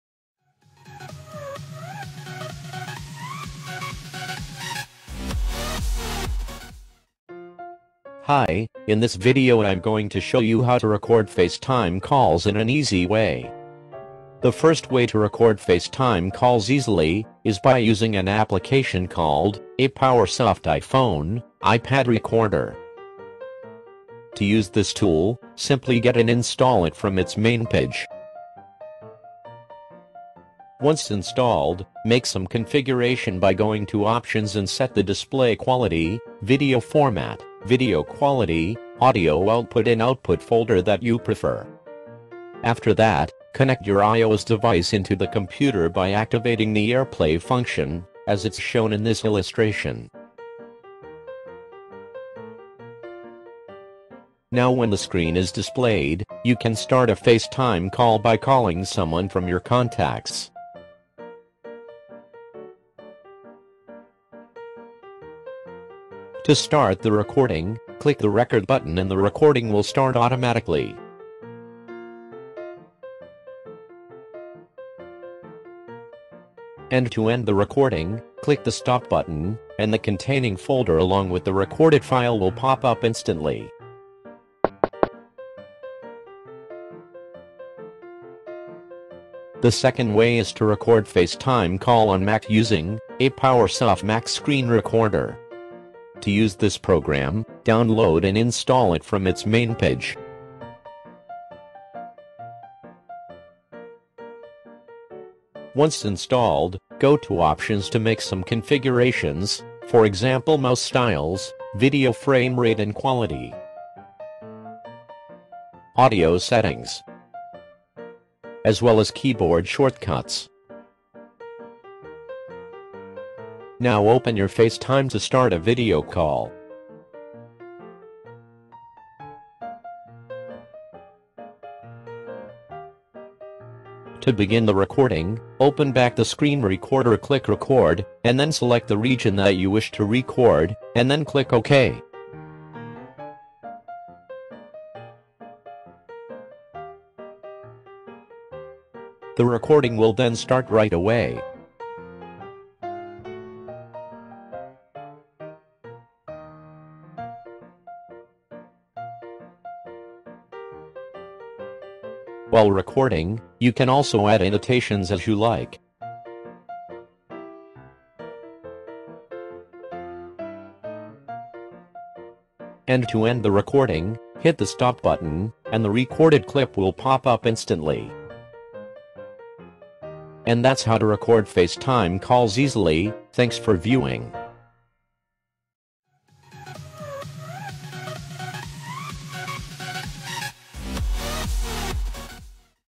Record FaceTime Calls in a Hassle-Free Way
Wanna record FaceTime on your Mac or iPhone/iPad? This guiding video shows you trustable programs that help capture a HD video call from FaceTime app. Just watch the video and you can learn more.